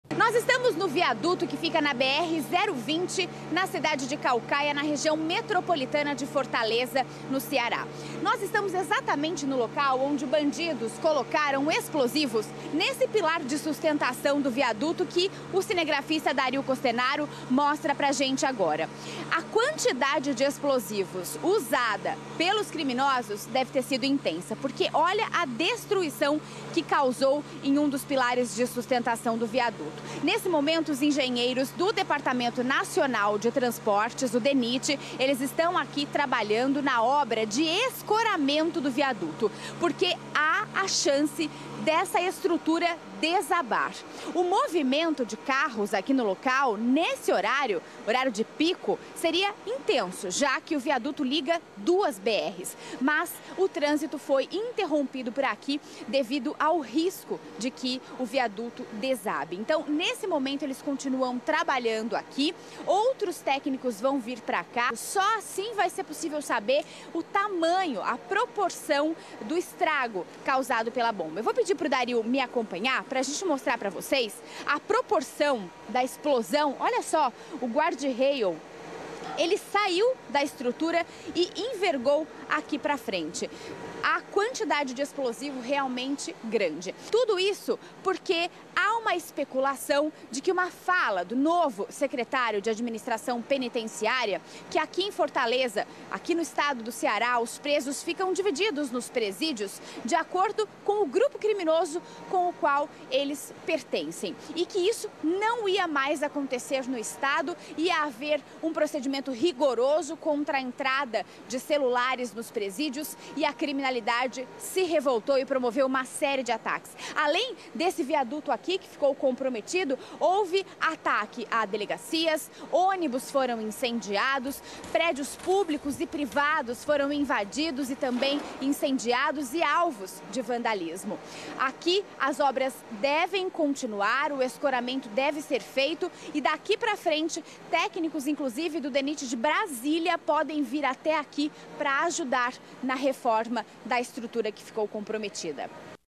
Bandidos explodem coluna de sustentação de viaduto no CE
O ataque faz parte das ações criminosas no Ceará que começaram após uma declaração do secretário estadual de Segurança Pública, que afirmou que os presos não ficariam mais separados por facções em presídios.